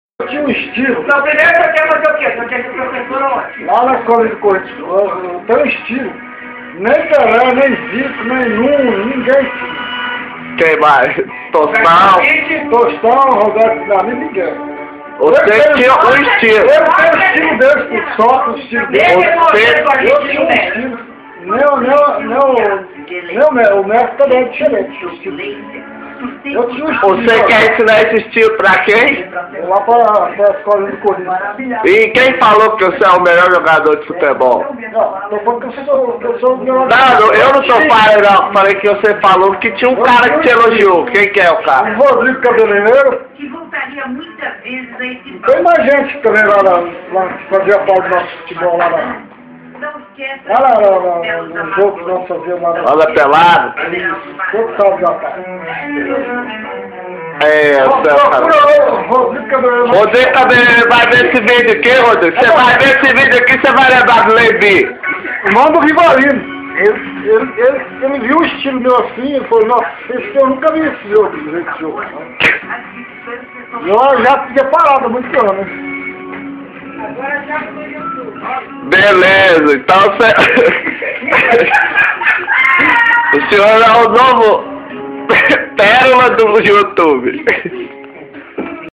Levi Melhor que o Messi
Levi tem o estilo de jogar futebol diferenciado melhor que o Messi, Roberto Dinamite, Pelé, Tostão, Socrates, Zico e Nunes dizia o Rodrigo Cabeleireiro